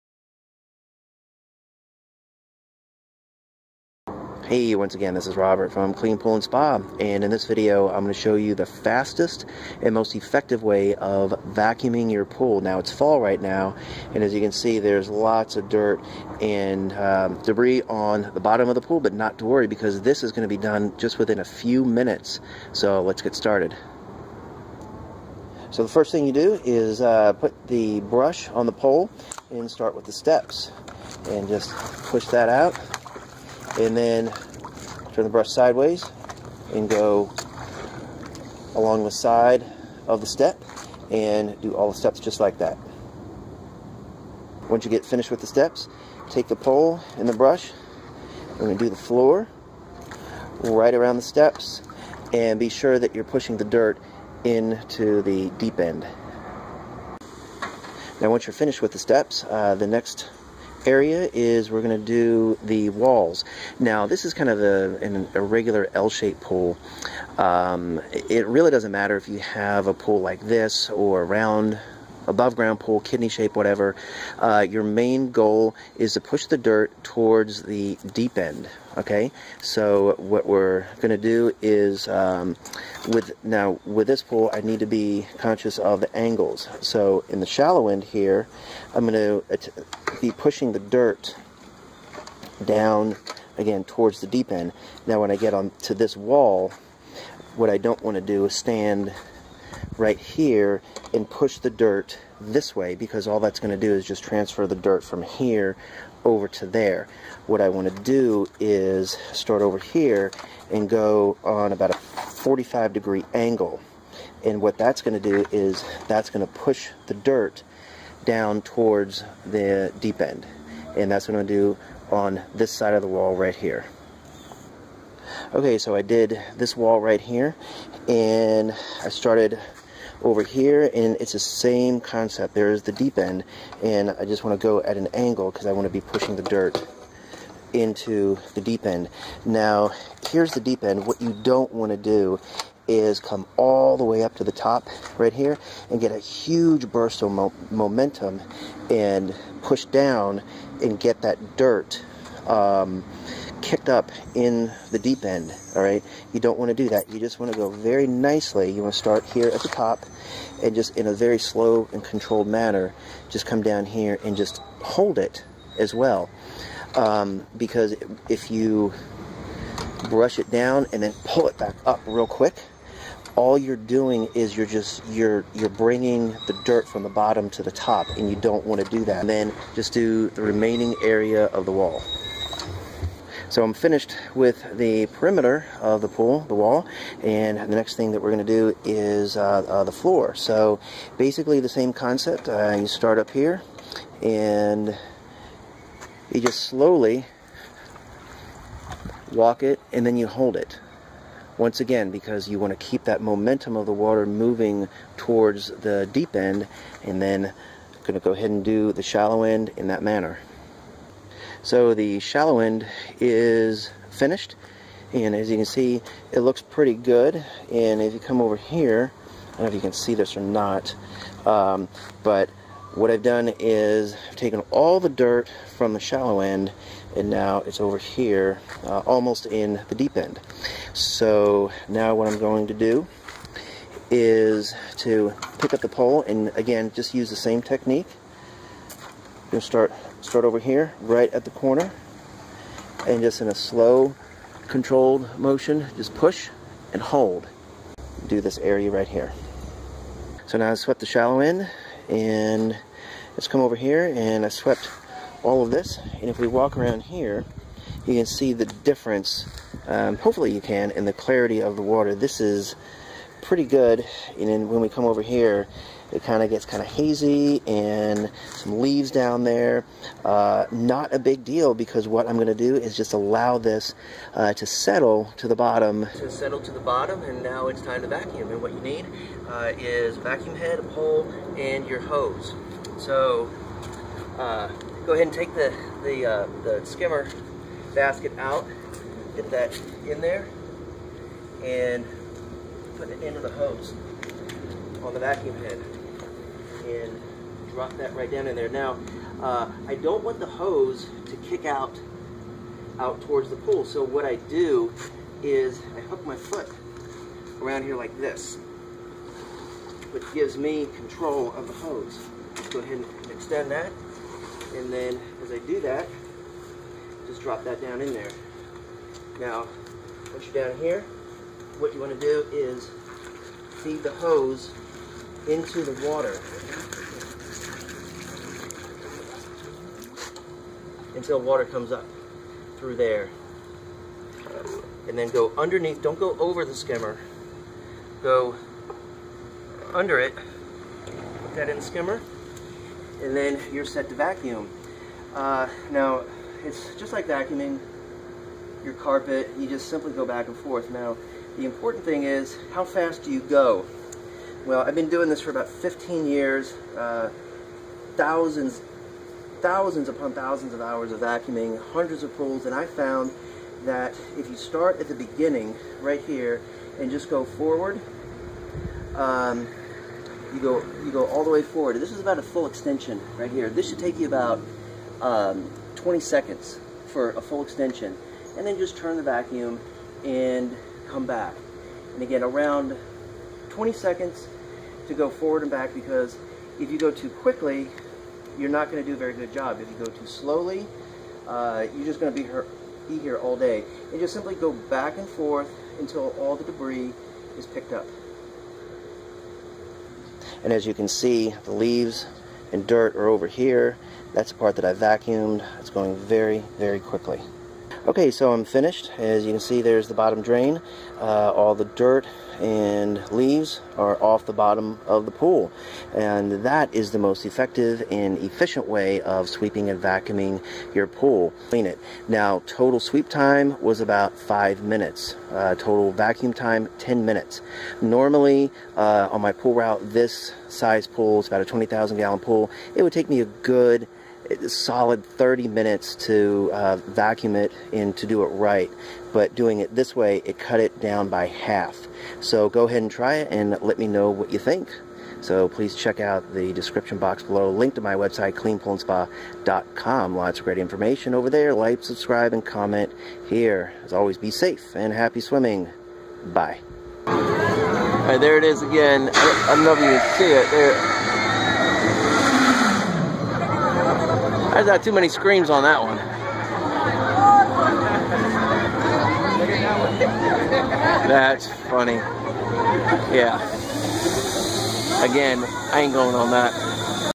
Fastest Easy Way To Sweep & Vacuum Swimming Pool | Dirt & Algae
Fastest way to sweep and vacuum your swimming pool - guaranteed.

*Sweep the steps and around the steps
*Sweep the walls. Be sure to get your angles correct. Sweep all the dirt and leaves towards to deep end.
*Sweep the bottom. All the dirt goes to the deep end.
*Allow the dirt and algae and debris to settle for a few minutes. Gravity will cause it to settle to the pool floor.

All of the pool's dirt should be collected in the deep end of the pool.

*Last step is to vacuum.

Be sure you have the filter OFF when sweeping. You don't want the dirt to hit any of the returns and be churned up.

This is the best and easiest way to get and keep your pool clean. You'll be able to cut the cleaning time down by 1/2.